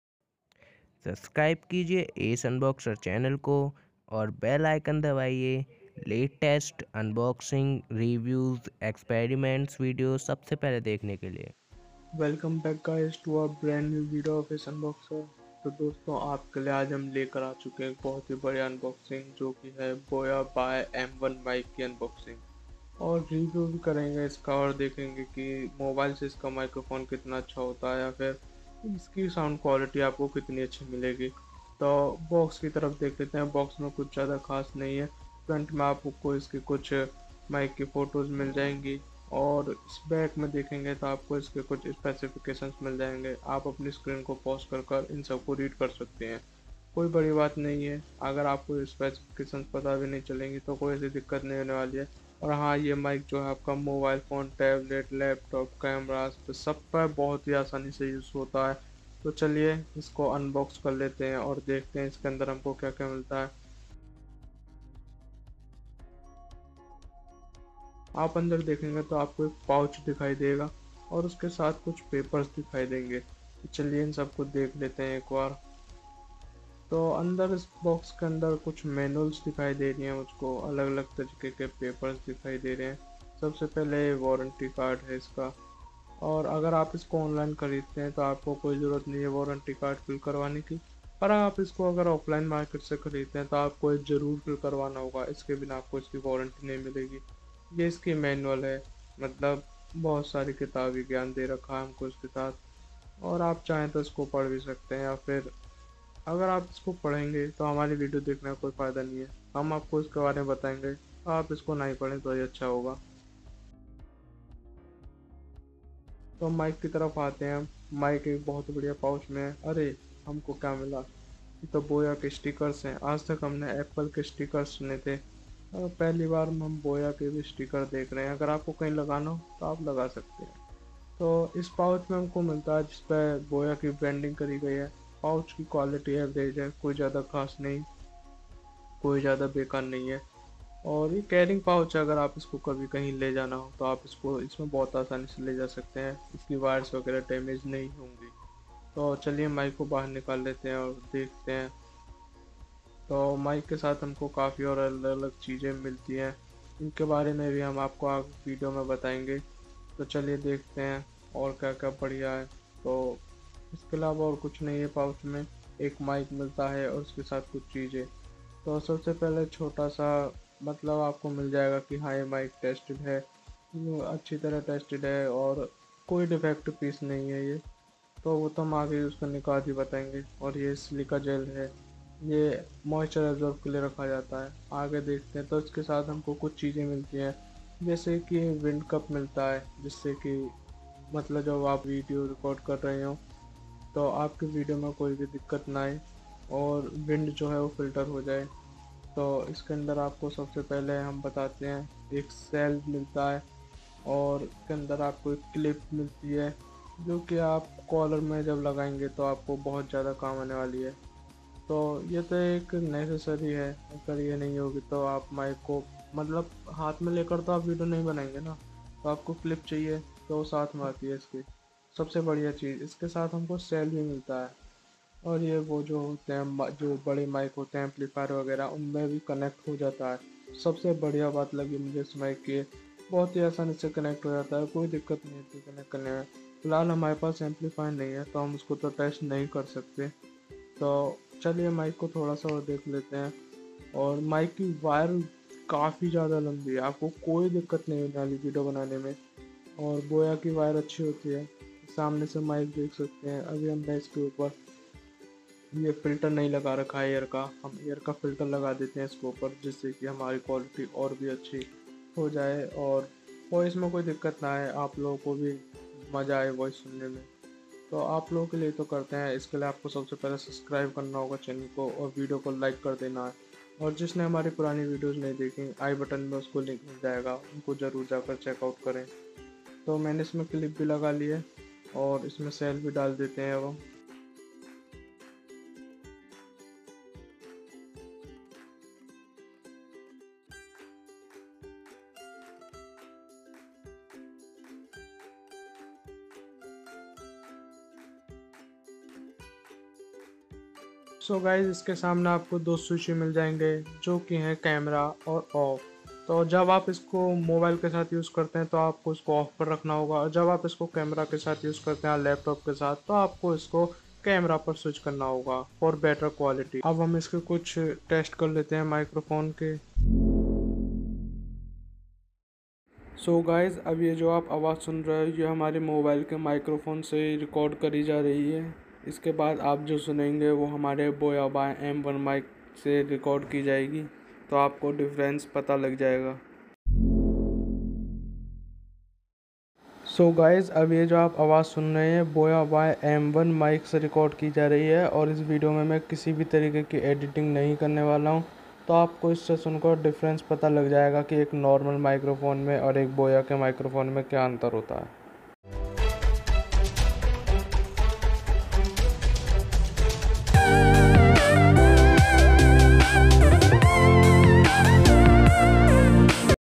Best Budget Collar Mic For Youtube Videos | BOYA BY M1 | Omnidirectional mic | ACE UUNBOXER
Hey Guyz,
                   In this Video We have unboxed and review boya by m1 mic for youtube videos creators. We have compared mobile microphone with boya by m1 mic.

MY GEARS:-

LaPTOP:- ASUS TUF GAMING F15
                  I7 10750H , GTX 1650TI